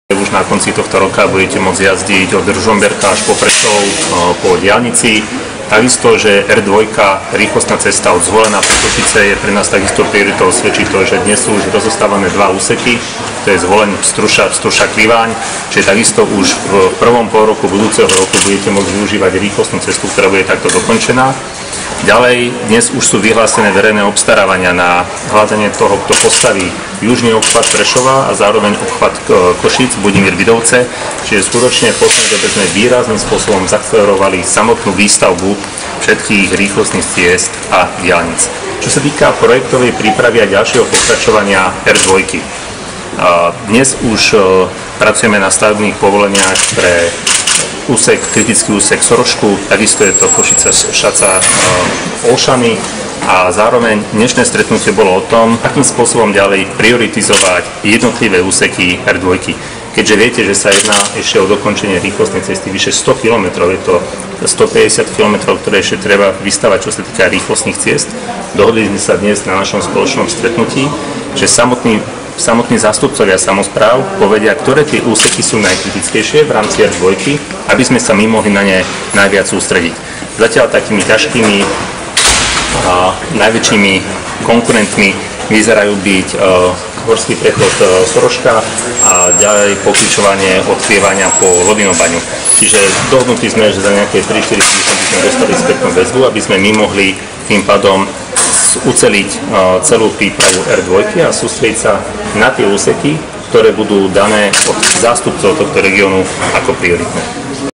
infoRoznava.sk Viktor Stromček o R2
Štátny tajomník ministerstva dopravy Viktor Stromček (SMER) o plánovanej výstavbe rýchlostnej cesty R2. Viac na Video: TASR